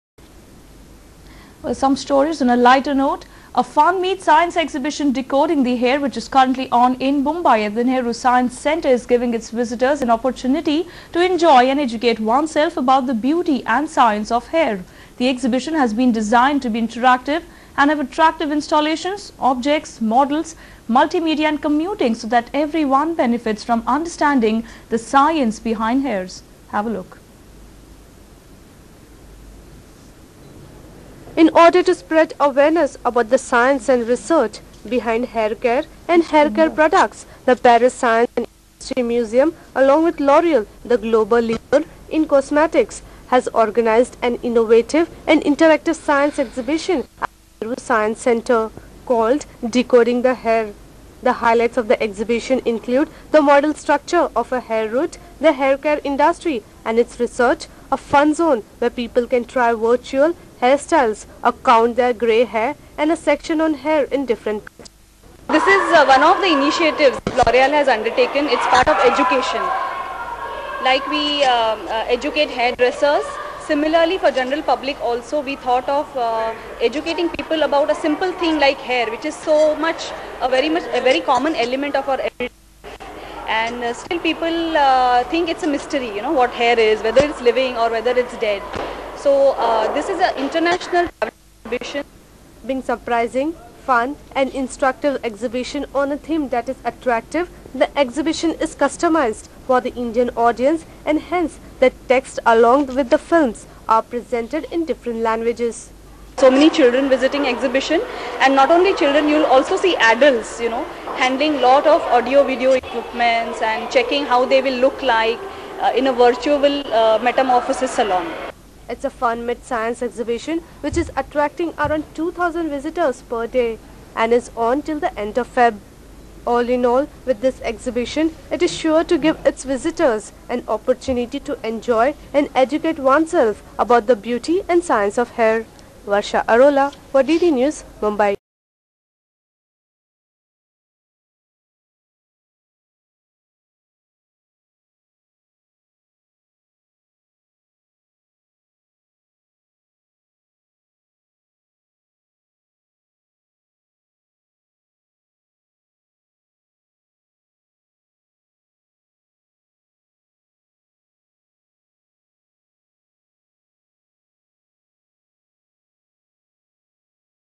Loreal - Decoding the Hair, DD News Coverage
"Decoding the hair" is a surprising, fun and instructive exhibition on a theme that is attractive by its familiarity, its universality, and its predominant place in our daily lives. It spans social sciences, life sciences, materials sciences, industry, technology and research. The exhibition has been designed to be interactive and will have attractive installations, objects, models, multi-media, computing it adapts to different places and cultures while retaining its coherence and legibility. The various cultural aspects of hair are also highlighted; 'Decoding the Hair' is a real scientific, aesthetic, historical, geographical and social voyage of discovery!

The exhibition will be customised for the Indian audiences and hence the text along with the films will be presented in four languages  Hindi, Marathi, English and English Braille. The exhibition is built around four sections:

1. Of life and matter
2. The science of the product
3. The metamorphosis salon
4. Hair of the world

The exhibition made a great success attracting three hundred and fifty thousand visitors when it premiered in 2001 at C.S.I. in France. Since then it has attracted one million and five hundred thousand visitors in London, The Hague, Lisbon, Berlin, Helsinki, Sao-Paulo, Mexico, Hong Kong, Beijing and recently at Russia (Moscow) and Taiwan (Taipei).

The centre is conveniently located between Mahalaxmi station and Worli Naka. The exhibition timing is 10 am to 6 pm and ticket counter timing is 10 am to 5 30 pm. The exhibition is open to one and all. The admission fee for general visitors is Rs 20 with discounts for groups and students. The Science Centre is open to public every day including Sundays and public holidays.

18 December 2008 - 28th February 2009

Venue:

Nehru Science Centre
National Council of Science Museums,
Dr. E Moses Road, Worli, Mumbai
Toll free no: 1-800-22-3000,

You can even connect us